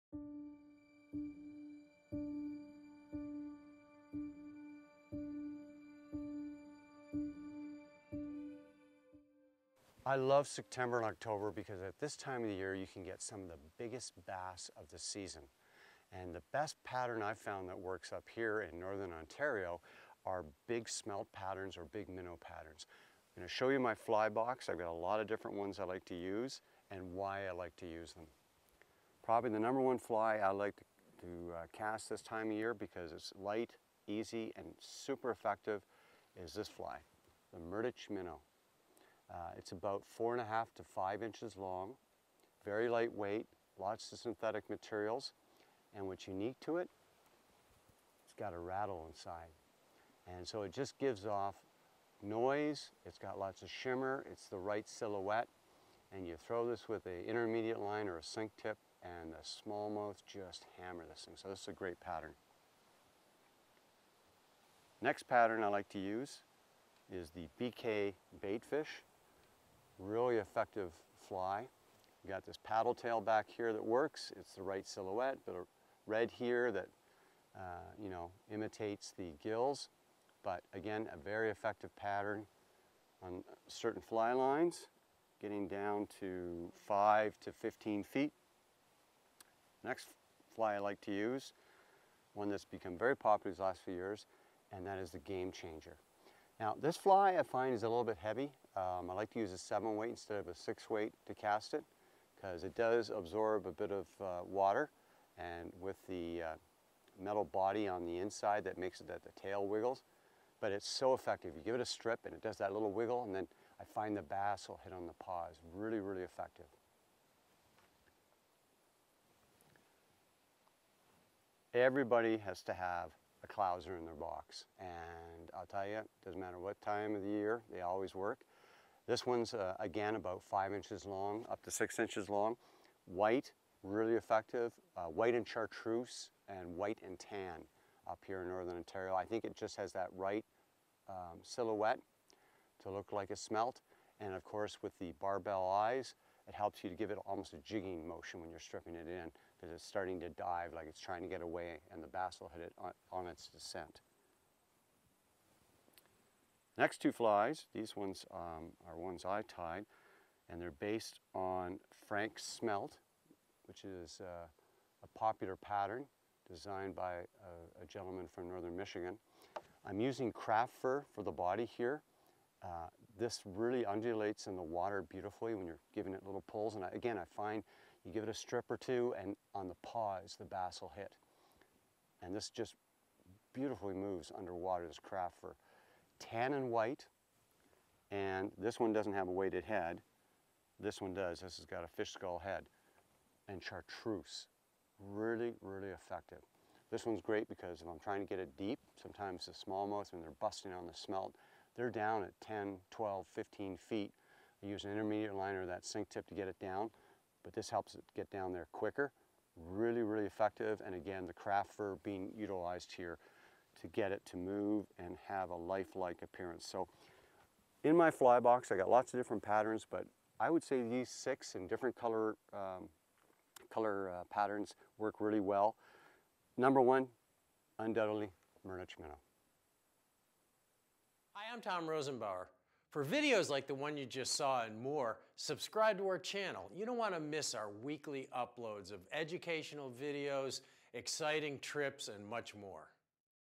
Smelt & Baitfish Patterns for Trout & Bass
Colin McKeown shows us his fly box of smelt & baitfish patterns and details his top producing patterns for both trout and bass fishing. These are excellent patterns for use on rivers and lakes throughout North America. They are exceptional for brook trout, lake trout, rainbow trout, smallmouth bass, brown trout, and largemouth bass. Every angler should have some of these patterns in their streamer box!